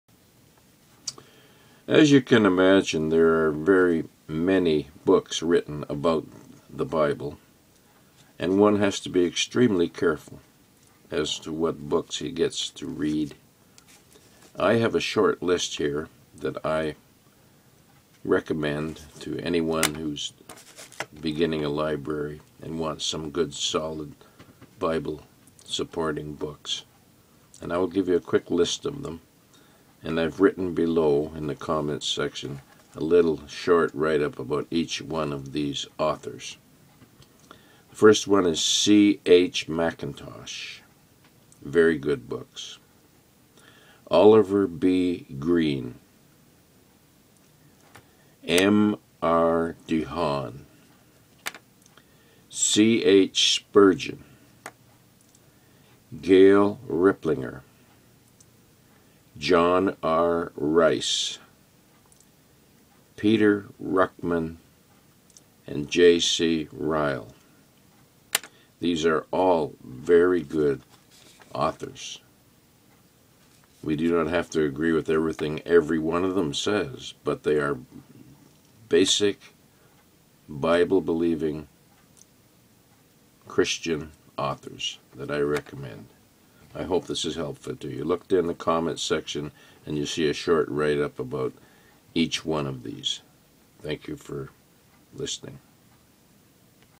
GOOD BOOKS ... GOOD AUTHORS
SOUND AUTHORS OF GOOD BOOKS ... I hope this is helpful to those wishing to obtain good sound books for their library. ( click on "show more" to read more about these authors )

Charles Henry Mackintosh was a nineteenth-century Christian preacher, dispensationalist, writer of Bible commentaries, magazine editor and member of the Plymouth Brethren. Born: October 1820, County Wicklow, Republic of Ireland Died: November 2, 1896, Cheltenham, United Kingdom

Oliver Boyce Greene (February 14, 1915 -- July 26, 1976) was a Independent Fundamental Baptist evangelist and author. He converted in 1935 at the age of 20. Over 200,000 confessions of faith were recorded as a result of his ministry. He attended North Greenville Baptist College briefly before entering full-time ministry, conducting revival meetings in tents and in churches across the eastern United States for 35 years. Greene wrote over 100 books and brief booklets about the Bible.

Martin Ralph DeHaan (March 23, 1891 - December 13, 1965) was a Bible teacher, the founder of the Radio Bible Class, and the co-editor of a monthly devotional guide Our Daily Bread.

Charles Haddon Spurgeon was a British Particular Baptist preacher. Spurgeon remains highly influential among Christians of different denominations, among whom he is known as the "Prince of Preachers". Born: June 19, 1834, Kelvedon, United Kingdom Died: January 31, 1892, Menton

Gail A. Riplinger
Gail Anne Riplinger is an American writer and speaker. Born 1947 Gail Riplinger is best known for her book "New Age Bible Versions." An Exhaustive Documentation of the Message, Men & Manuscripts Moving Mankind to the Antichrist,

John Richard Rice was a Baptist evangelist and pastor and the founding editor of The Sword of the Lord, an influential fundamentalist newspaper. Born: December 11, 1895, Texas, United States Died: December 29, 1980

Peter Sturges Ruckman is an American Independent Baptist pastor. He is also the founder of the Pensacola Bible Institute, in Pensacola, Florida. Born: November 19, 1921 (age 92), Wilmington, Delaware, United States Education: University of Alabama, Bob Jones University

John Charles Ryle was the first Anglican bishop of Liverpool. Born: May 10, 1816, Macclesfield, United Kingdom Died: June 10, 1900 Education: Christ Church, Oxford, Eton College, University of Oxford Children: Herbert Edward Ryle
THE BOOK I'M READING NOW is by William Gurnall - "The Christian in Complete Armour." C.H. Spurgeon: said "Gurnall's work is peerless and priceless; every line is full of wisdom; every sentence suggestive. The whole book has been preached over scores of times, and is, in our judgement, the best thought-breeder in all our library." Here's a quote from this author - "Humble souls are fearful of their own strength."

Here are some good teaching preaching videos
( Three 15 minute videos )